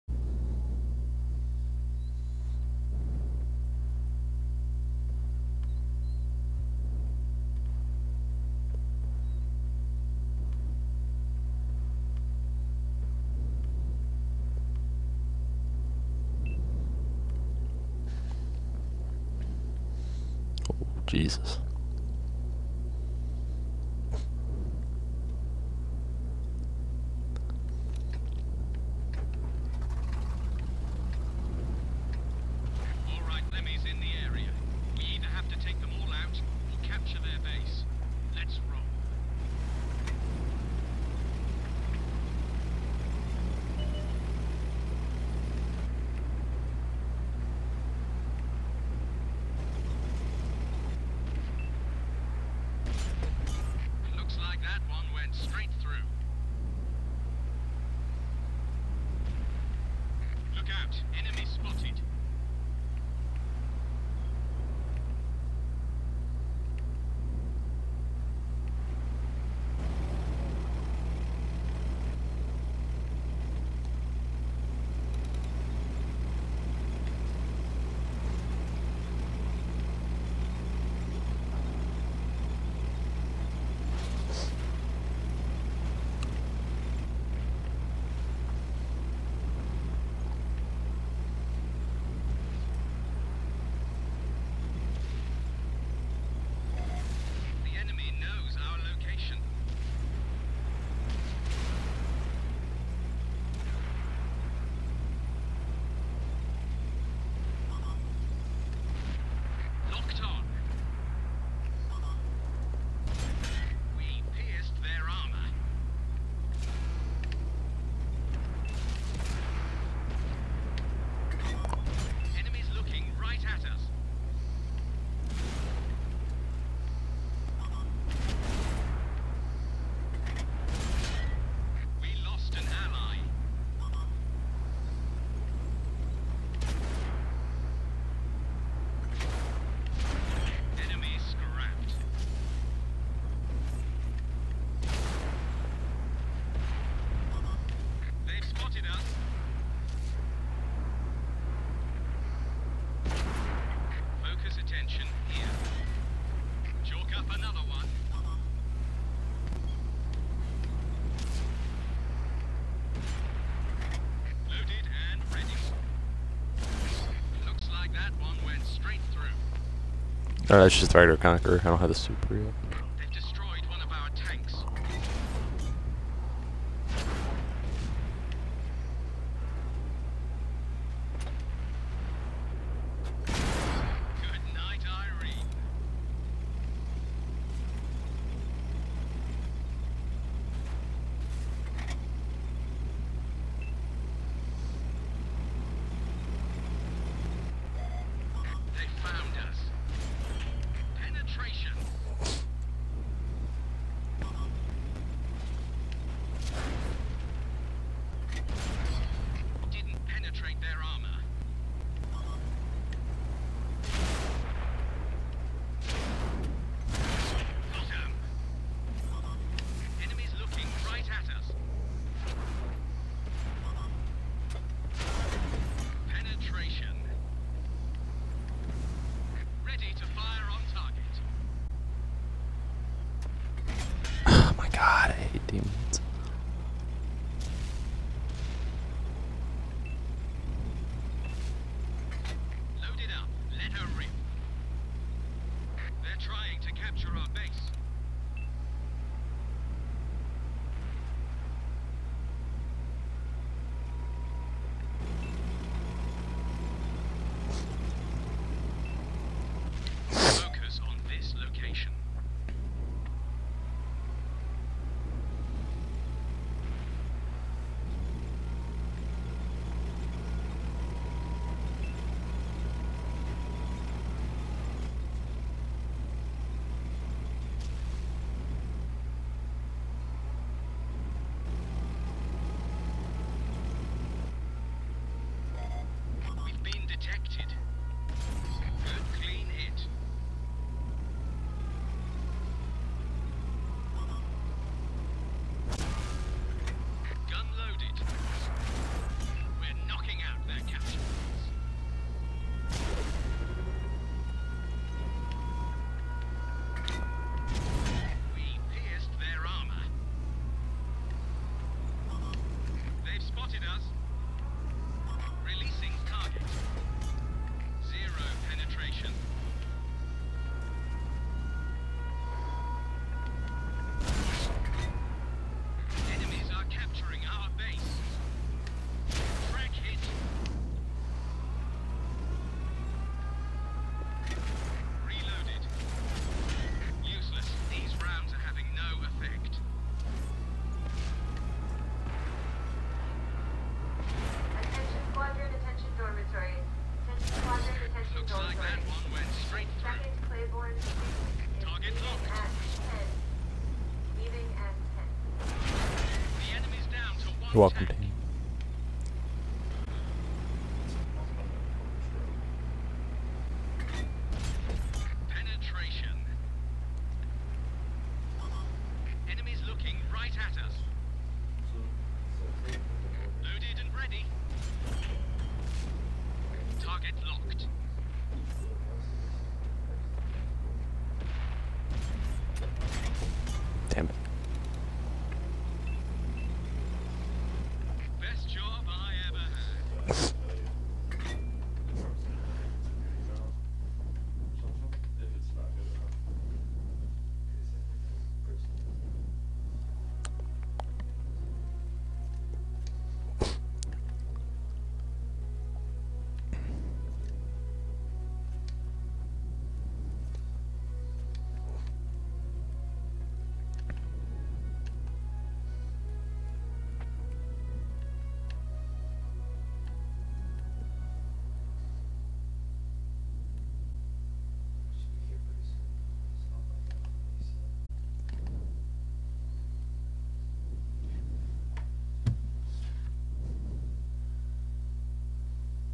A Path of Destruction: Conqueror Gameplay
Like many I want to get the Super Conqueror, but the grinding for me at the moment is rather slow. But I can deal with it now that the tier 9 is improved as well. Making it a much more enjoyable tank for myself to play even if it is a slow heavy tank. I'm still a little surprised at how I didn't die in the end since I was merely trying to reset once or twice before dying but I had other plans apparently.